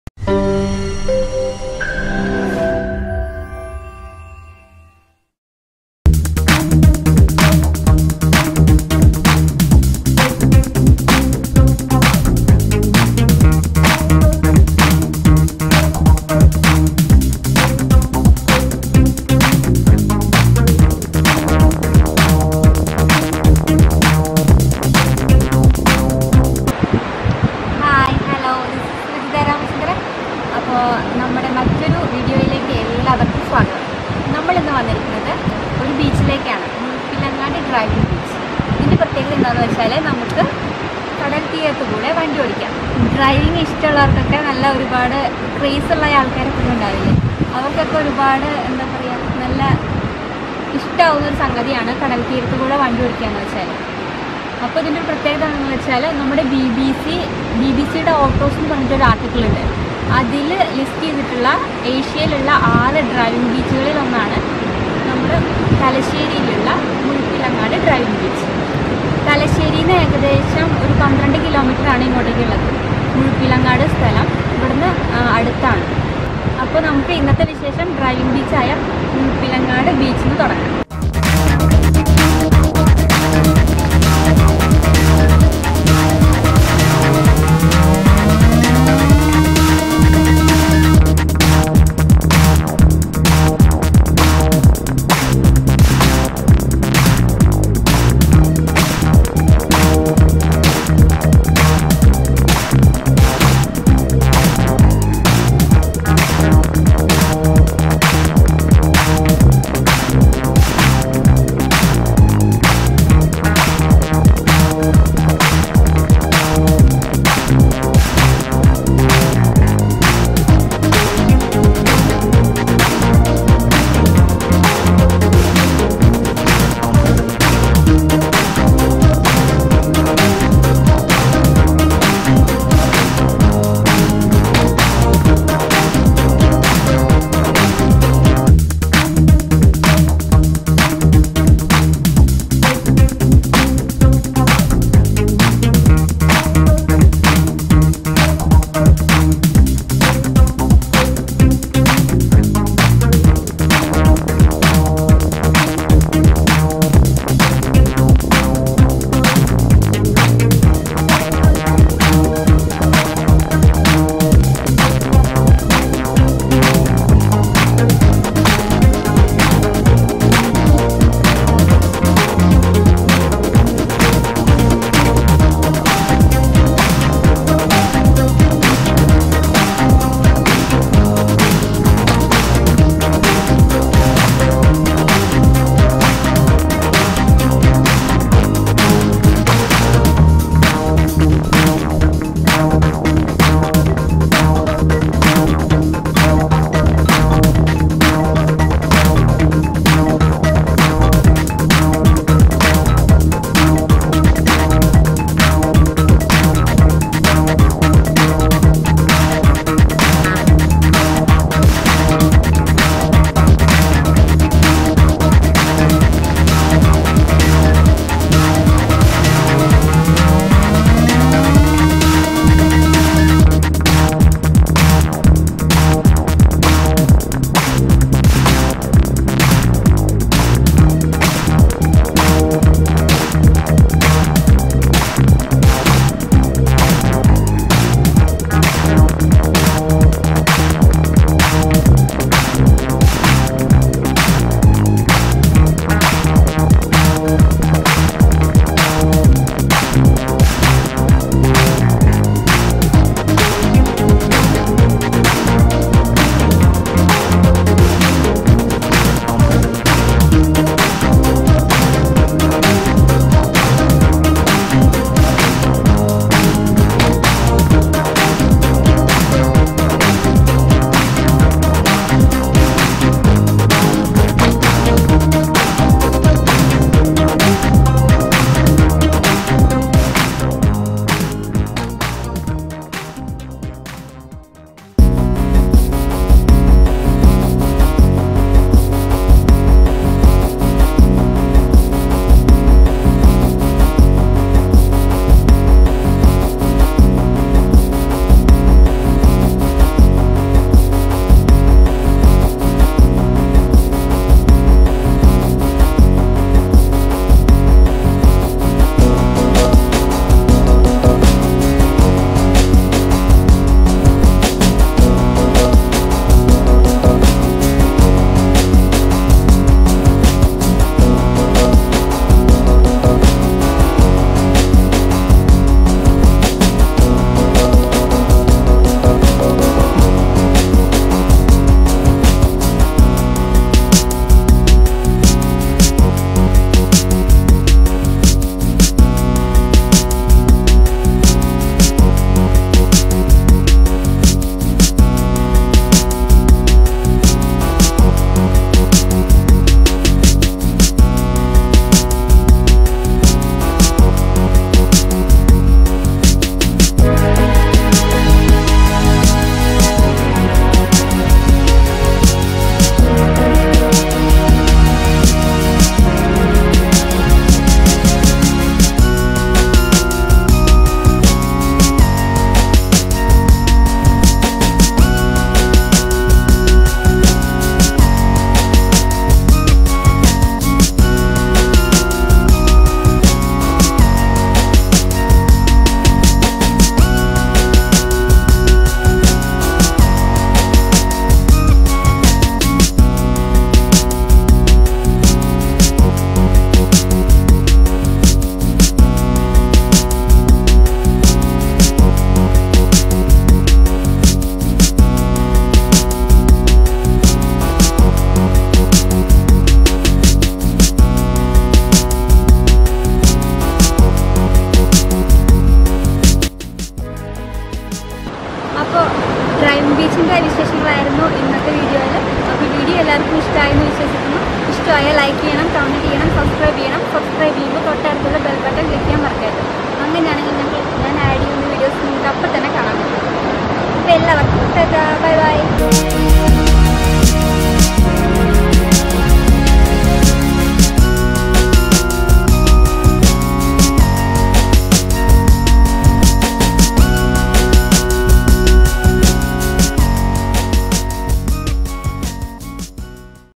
Muzhuppilangad Drive-In Beach | മുഴുപ്പിലങ്ങാട്
Muzhuppilangad Drive-In Beach is longest Drive-In Beach in Asia and is featured among the top 6 best beaches for driving in the world in BBC article for Autos.